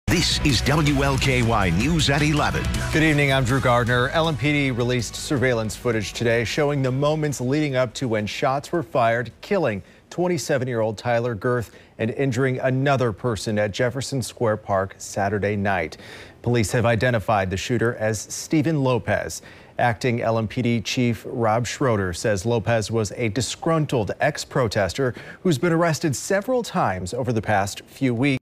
Suspect in Jefferson Square shooting had caused trouble at protests before, LMPD says
Officials have identified the man killed in a double-shooting in Jefferson Square Saturday night.